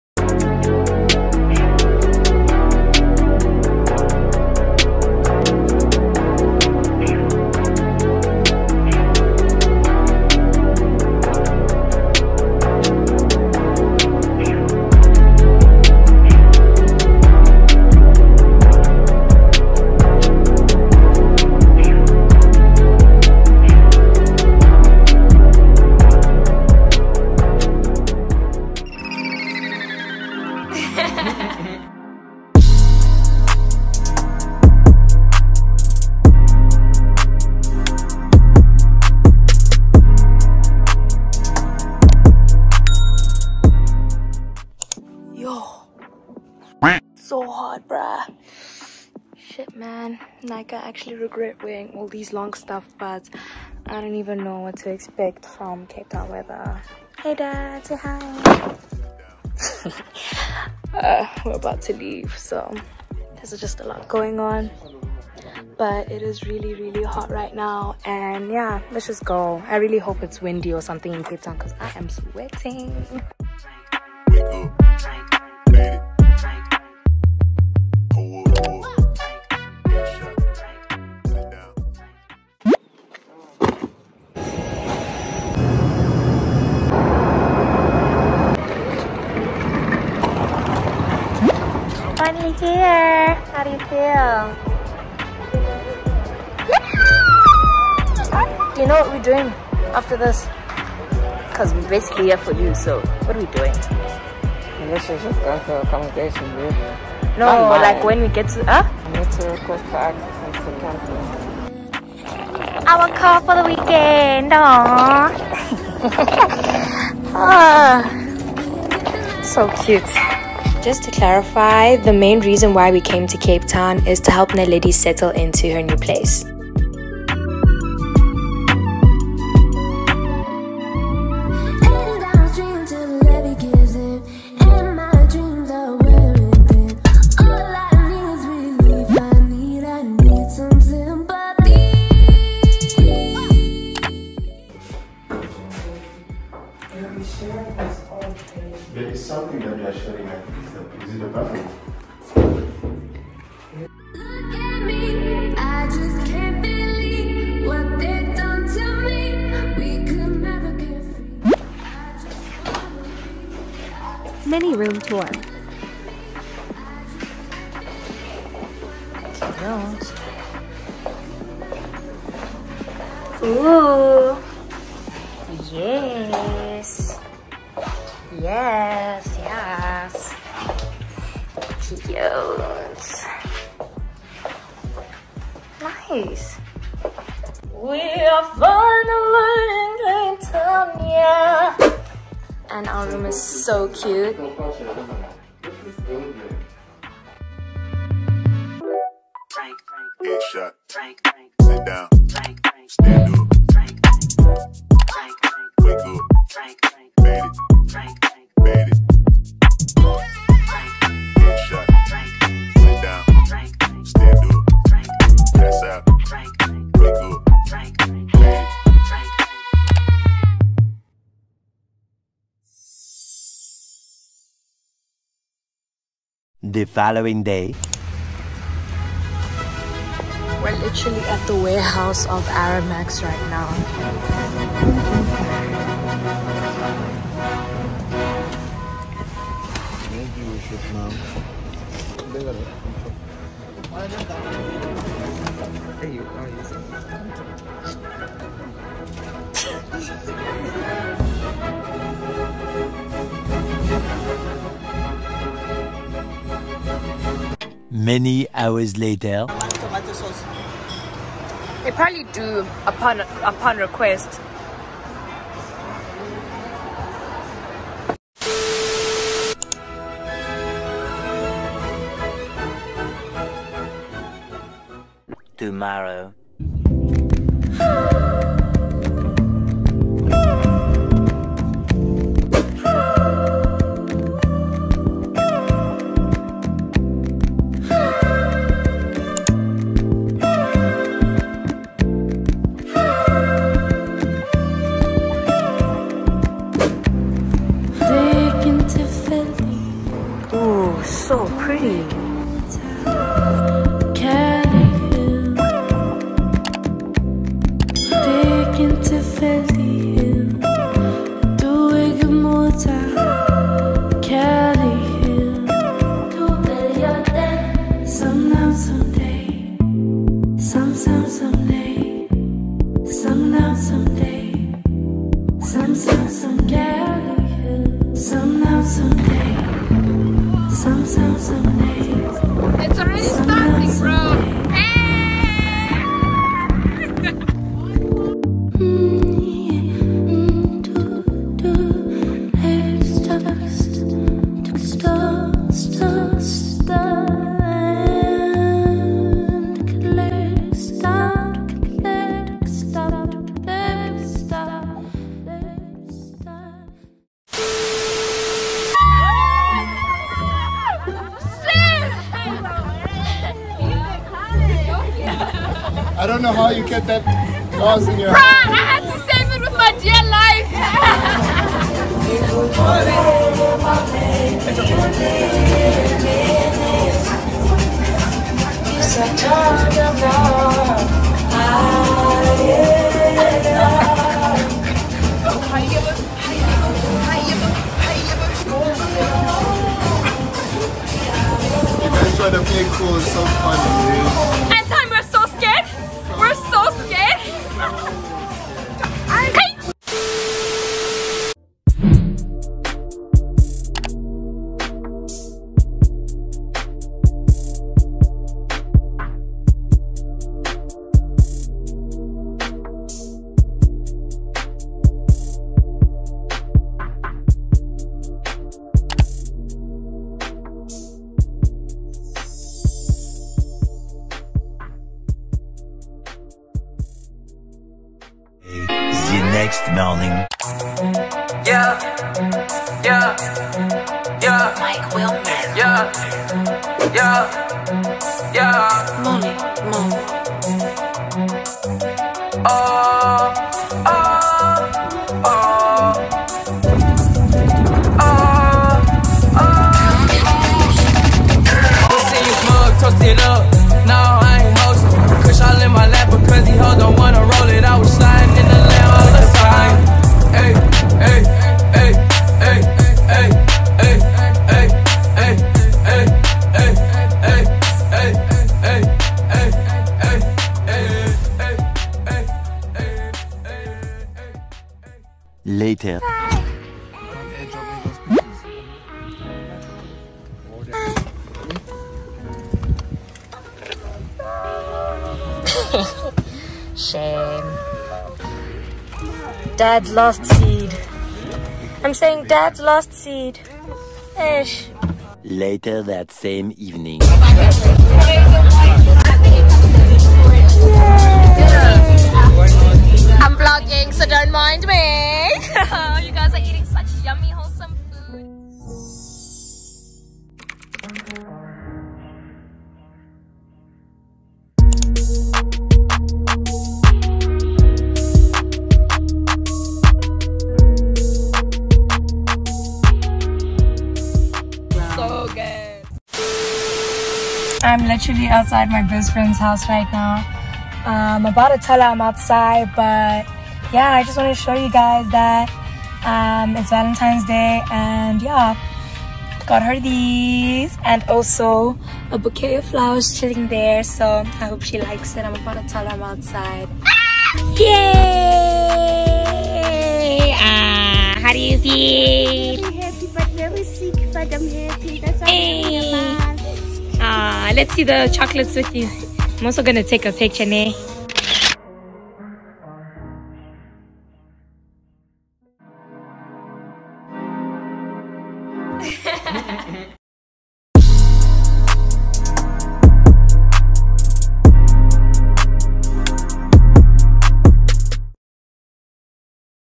CAPE TOWN VLOG: Wine Farm, Sunset Cruise, Atlantis Dunes +more | SIHLE MHLABANE
*OPEN AND WATCH IN HD 1080PX*

Hey Rockstars🤪
In tonight's video, you'll be watching me and my family head down to Cape Town to help my lil sis move into her new place as she just started with her tertiary studies. And since we haven't been to the Western Cape in a minute, we also toured around the area and visited a wine farm, went on a sunset cruise and went to Atlantis Dunes. I hope you enjoy the video as much as we had fun travelling to Cape Town. 🥺

DON'T LEAVE JUST YET...LET'S CATCH UP !

filmed by: Sihle, Thabang and Naledi Mhlabane
edited by: Sihle Mhlabane
camera: iPhone 12 Pro Max
Editing Software: VLLO, iMovie & Capcut